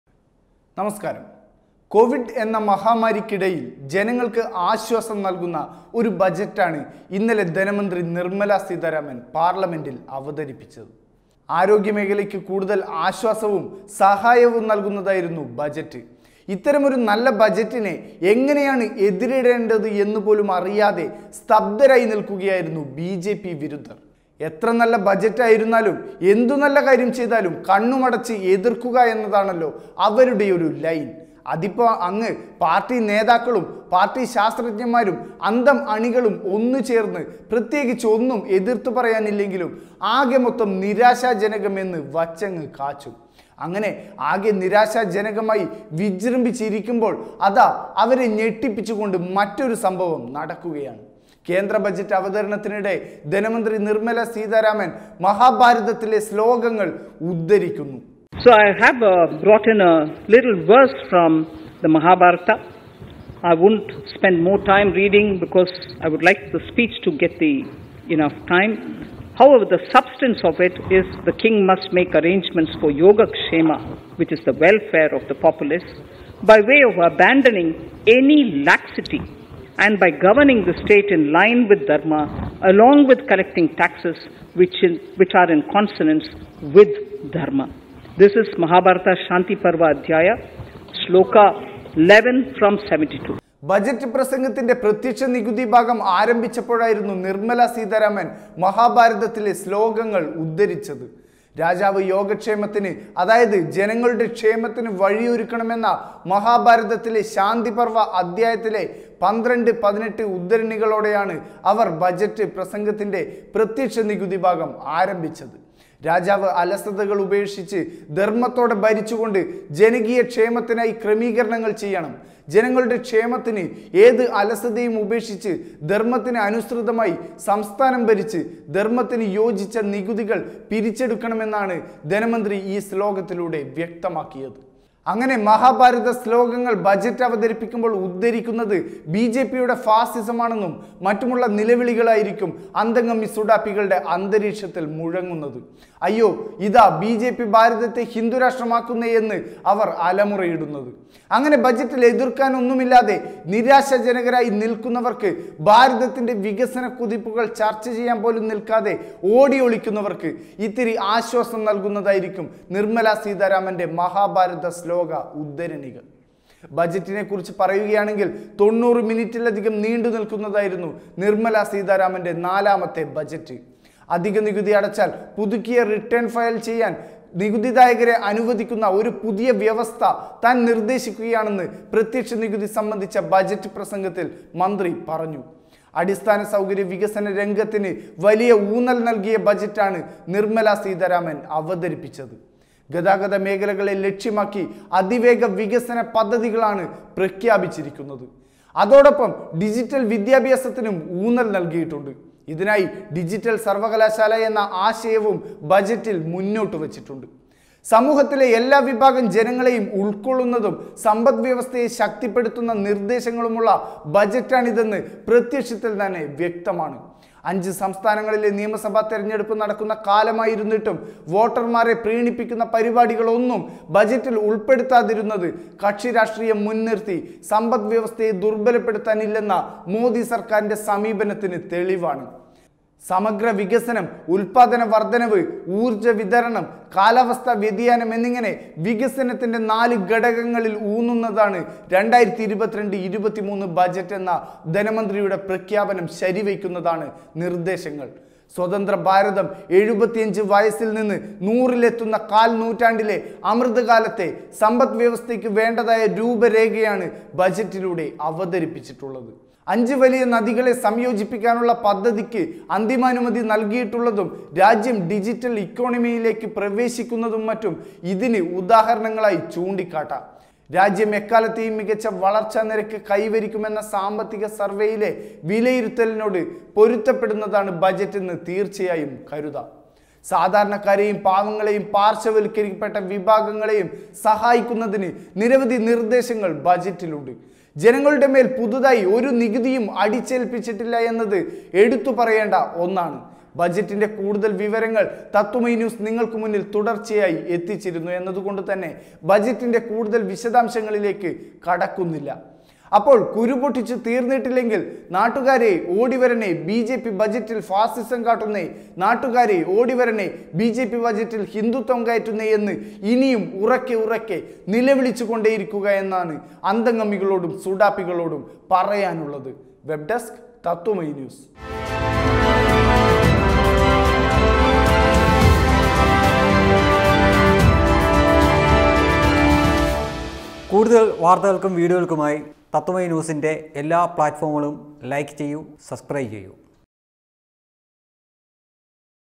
നാട്ടുകാരേ ഓടിവരണേ, ബിജെപിയുടെ ഫാസിസം ബജറ്റിലും | Mahabharatham
ബജറ്റിനിടെ മഹാഭാരതശ്ലോകം ഉരുവിട്ട് ധനമന്ത്രി, ചങ്കുപൊട്ടി നിലവിളിച്ച് ഹിന്ദു വിരുദ്ധർ | Nirmala Seetharaman